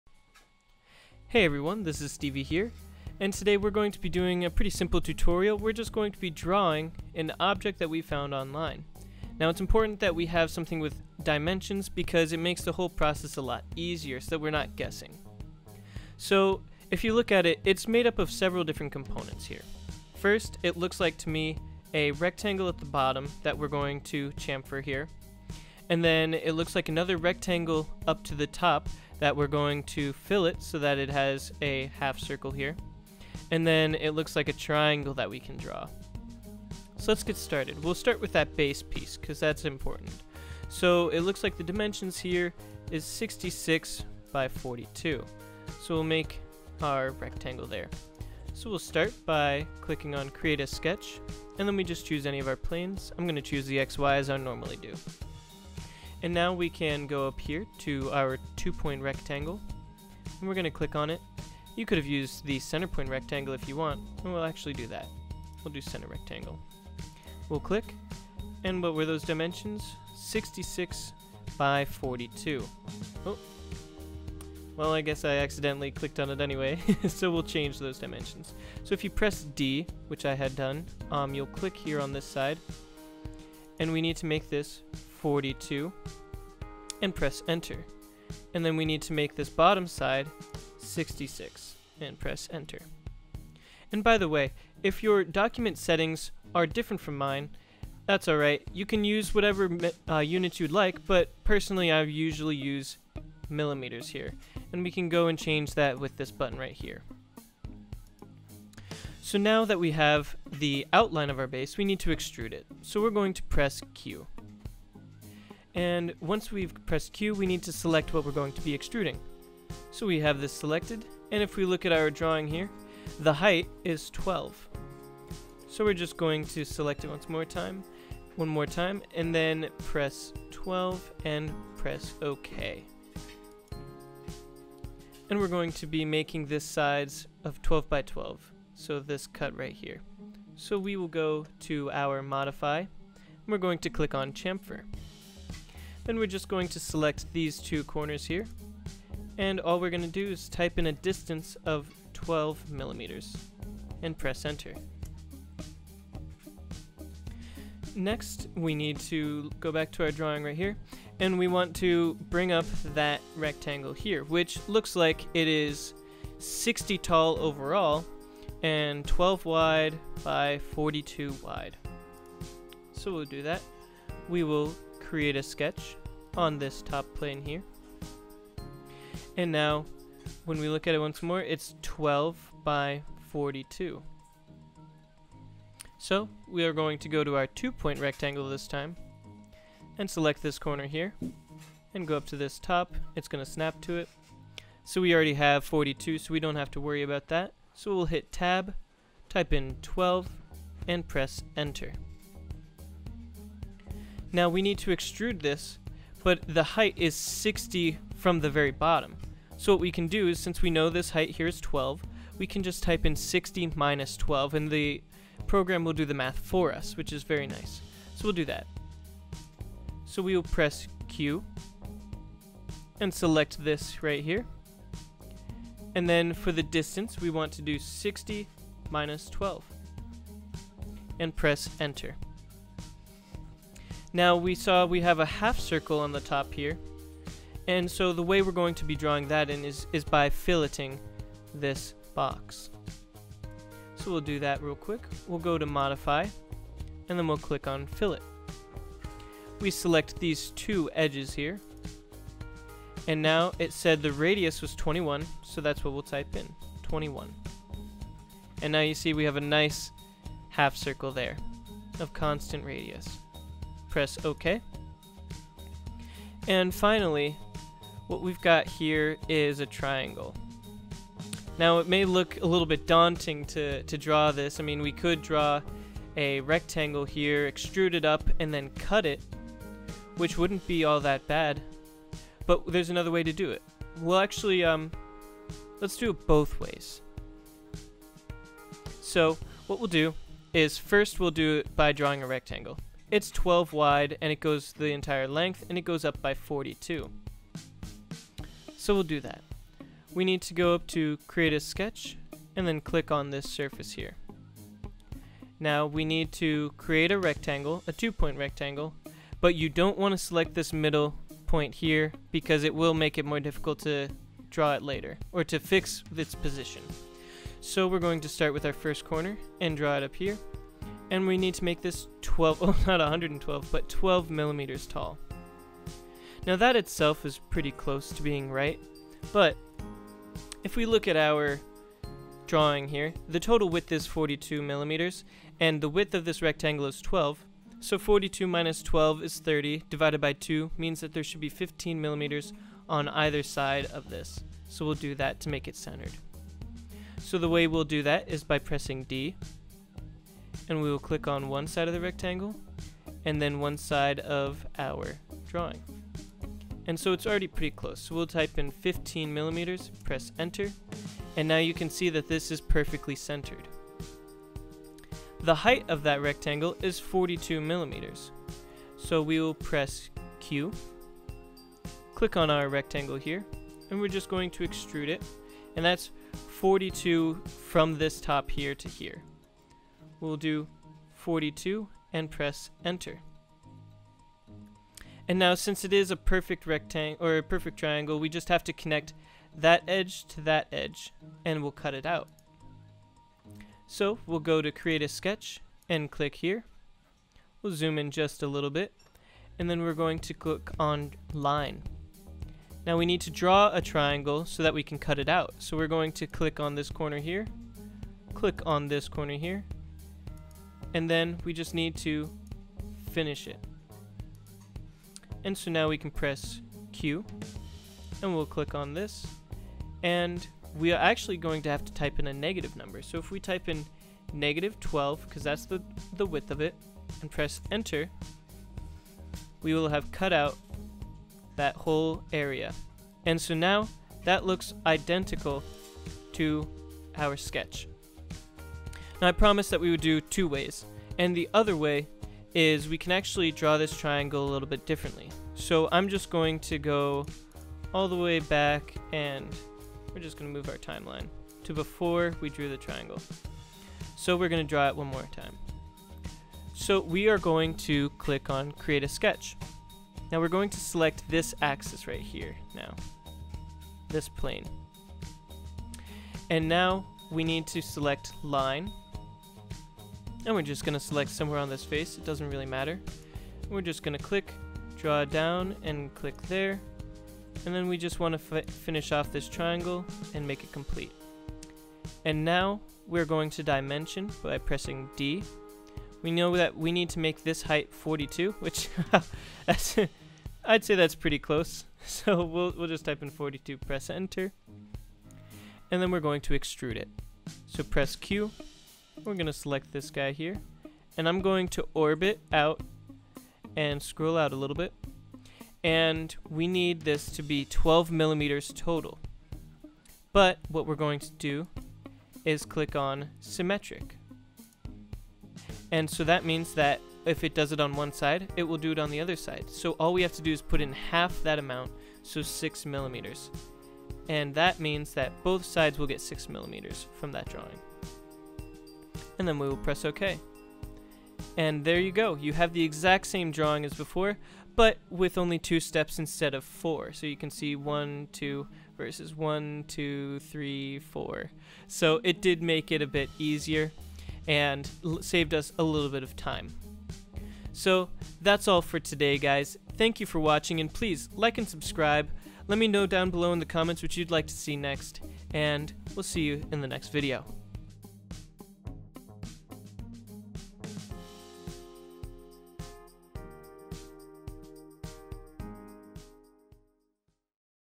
Fusion 360: Beginner tutorials part 1
Here we are seeing how we can take a dimensional drawing and turn it into a 3D object that we can even print.